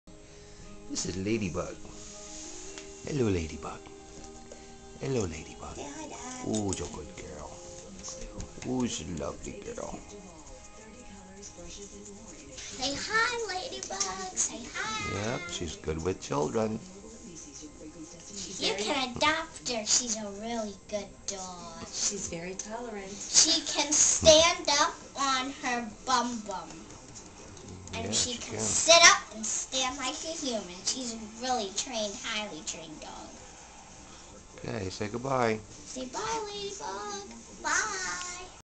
Lady
Lady (or Lady Bug) is a sweet soft cuddle-bug looking for a forever home.